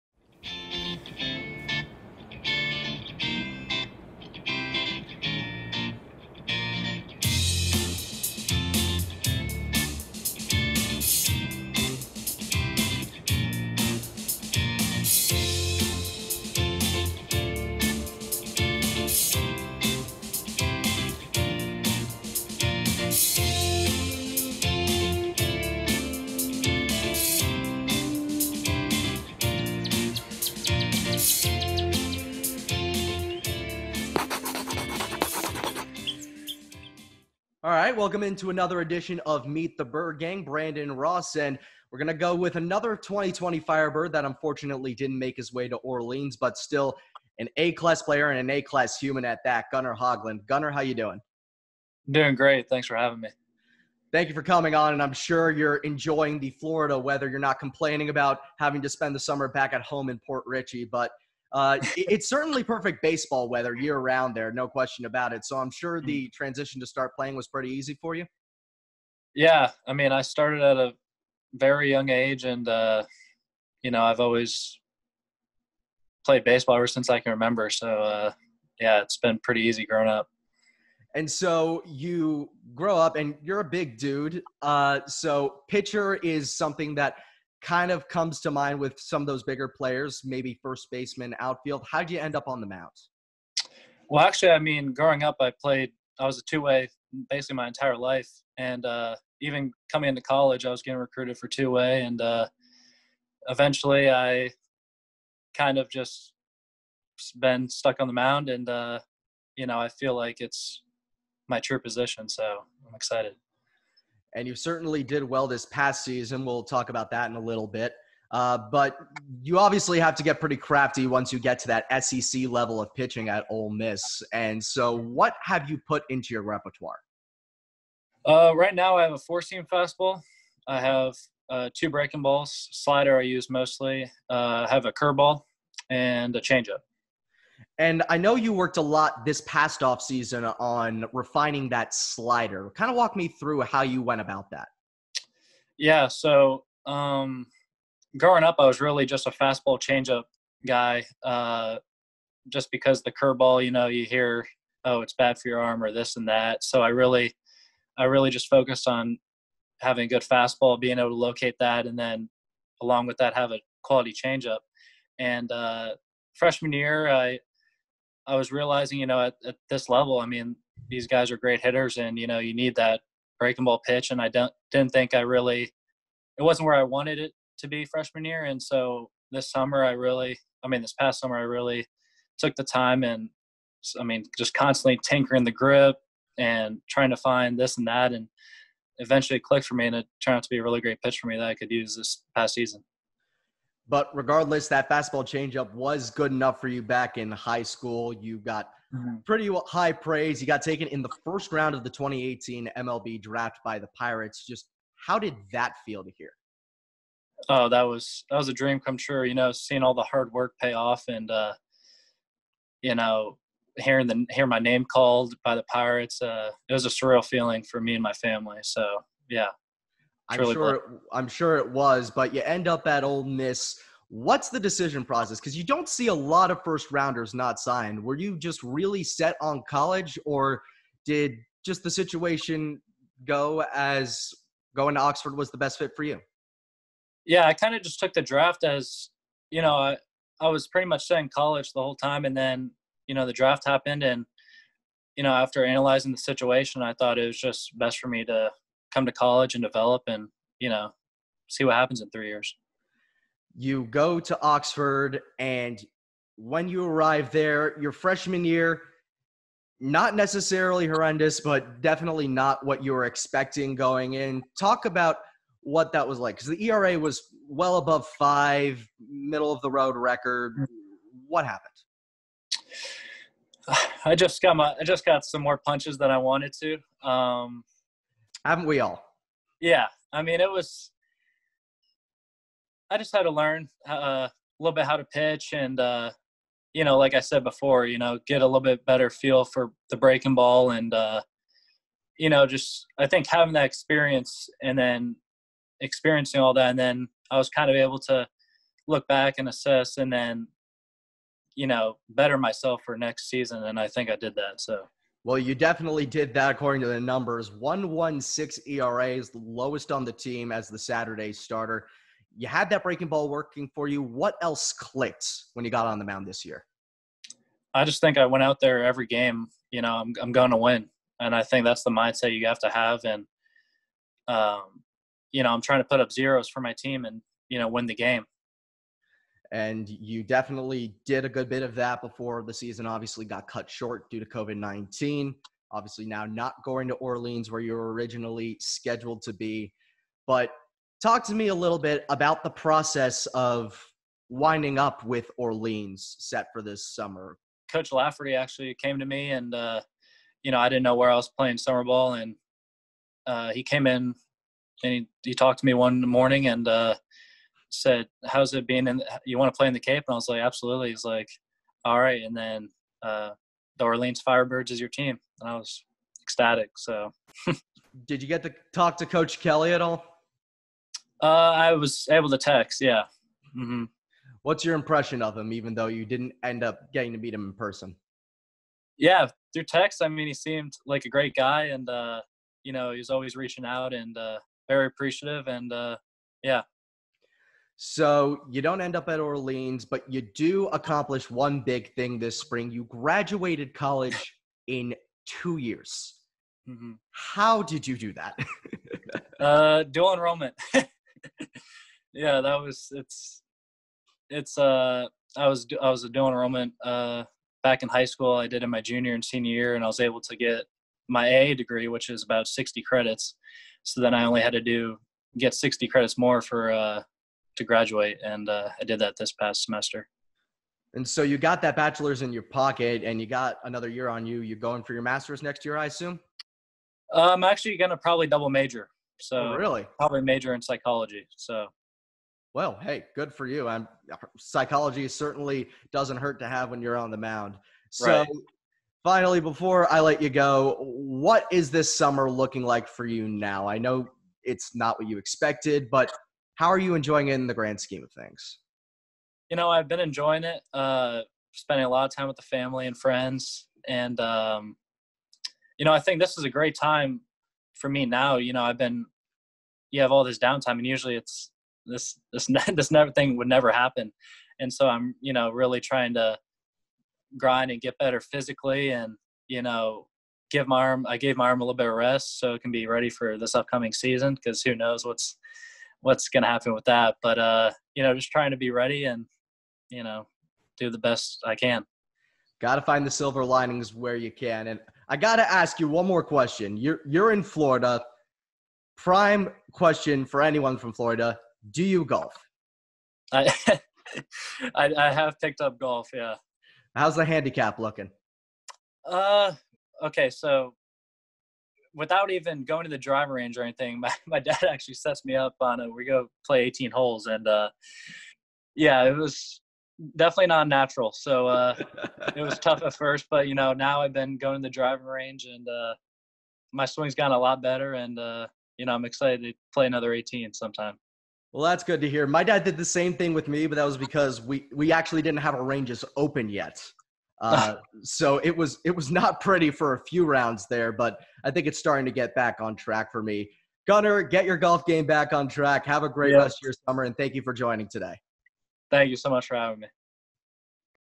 Episode 20 of Meet the Bird Gang- Gunnar Hoglund
On this Episode of Meet the Bird Gang, Brandon Ross chats with Gunnar Hoglund about:

-Why he stood by his commitment to Ole Miss despite being a 1st Round Pick
-What he changed that helped him improve his ERA by more than 4 points in his sophomore year
-Graduating college in 2 YEARS